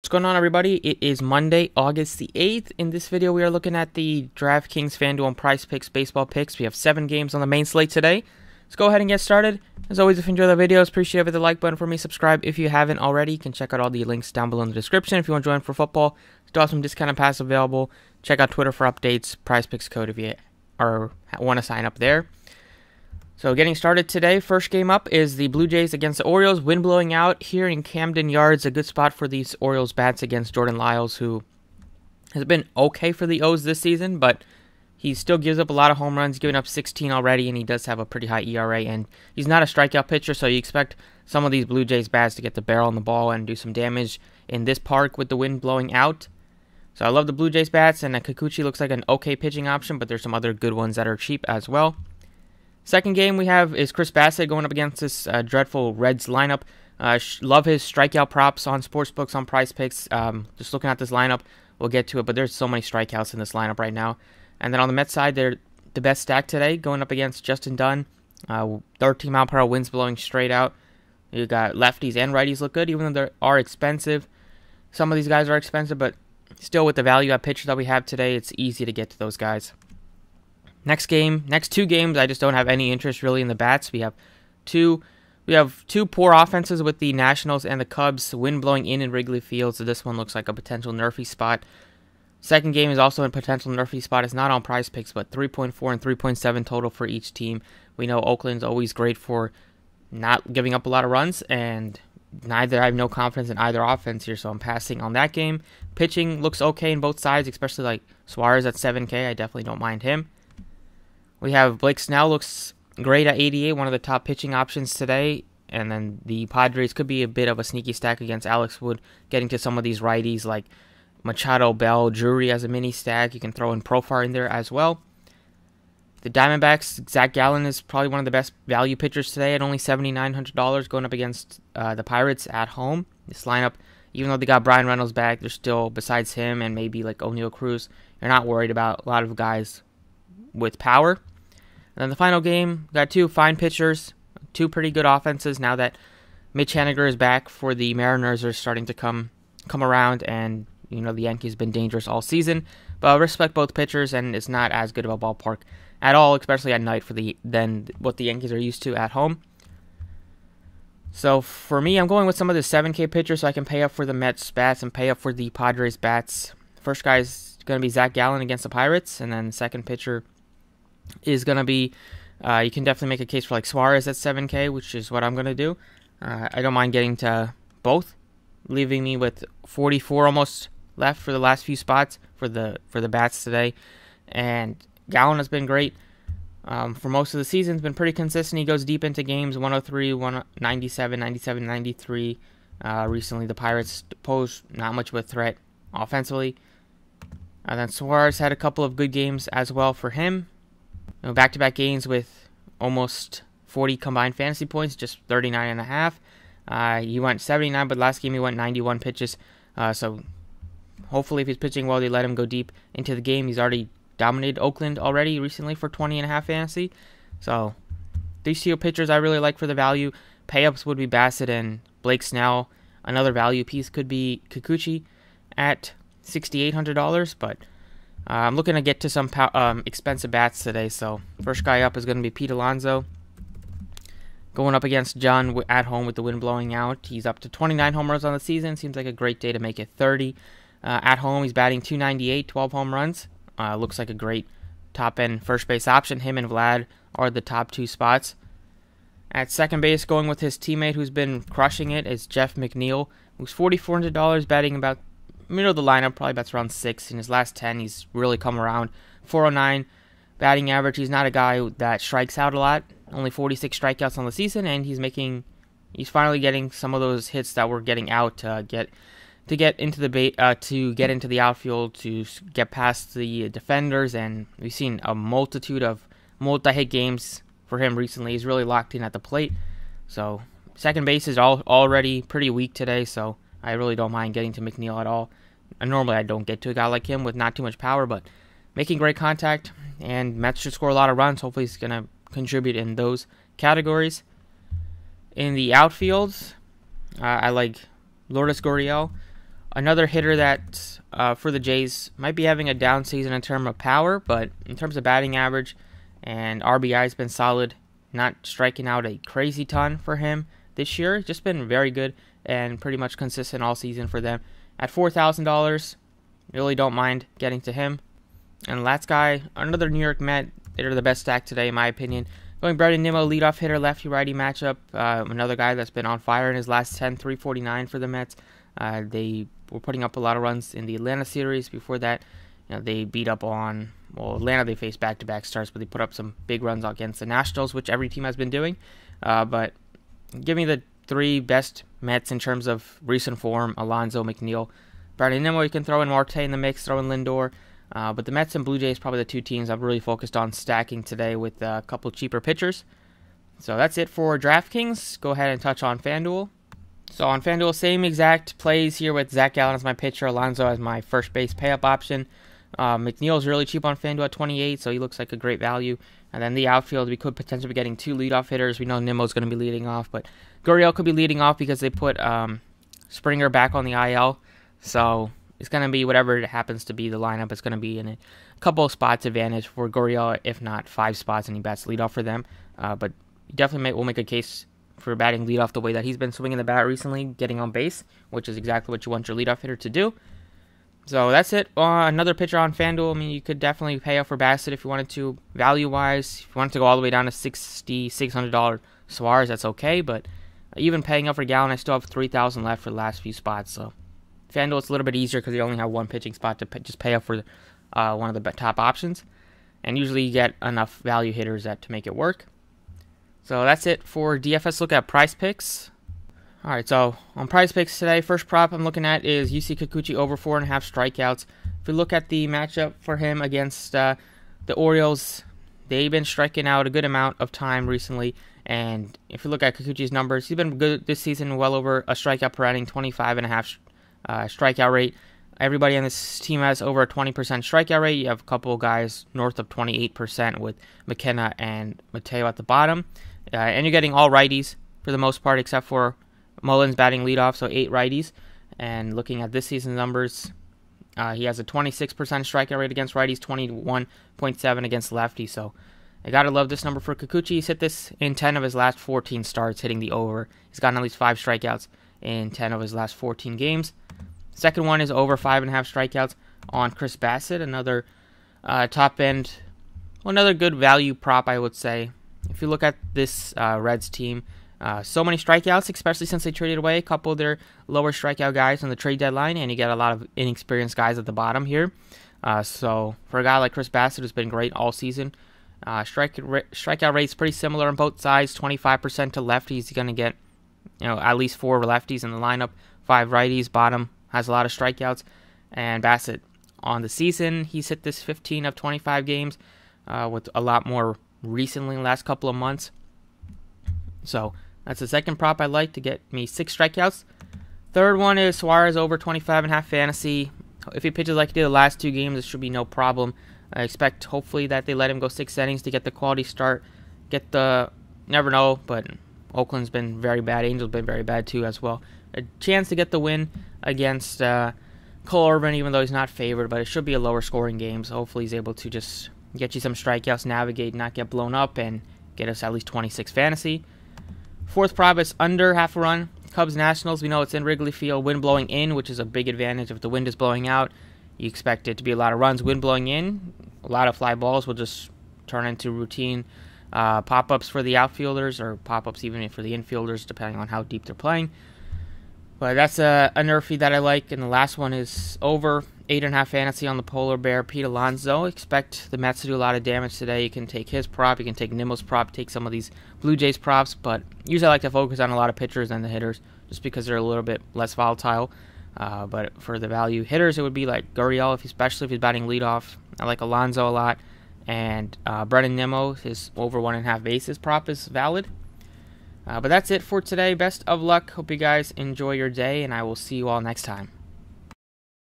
PRIZEPICKS DRAFTKINGS & FANDUEL 8/8/22 MLB TOP PLAYS
Here are my 8/8/22 MLB Top PrizePicks Draftkings & Fanduel picks!
PrizePicks: code "process"